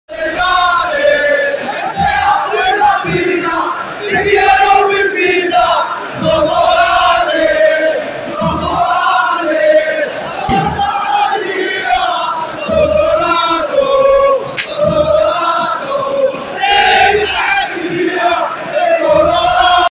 Craquage RCA Vs MAS : BOTOLA pro 2022/2023
fumigène et craquage ambiance RCA Vs MAS BotolaPro